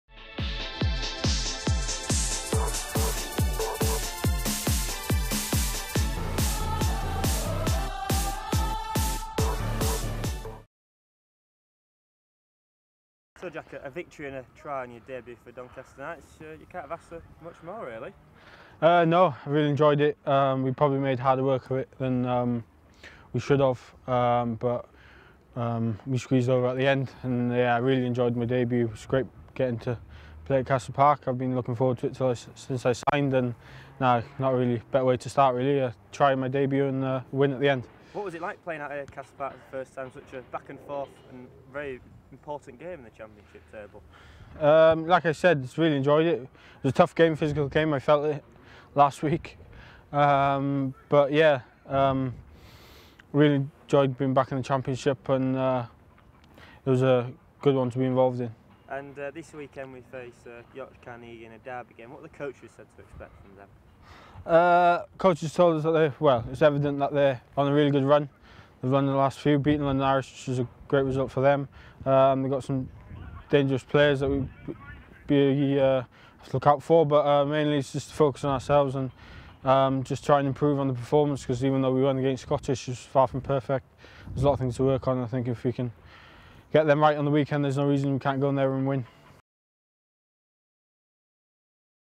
DKTV | Jack Roberts: 'No reason we can't win at Carnegie.'
Jack Roberts delighted with his debut after scoring a try as the Knights secured a five point victory and is confident a victory can be claimed against Yorkshire Carnegie.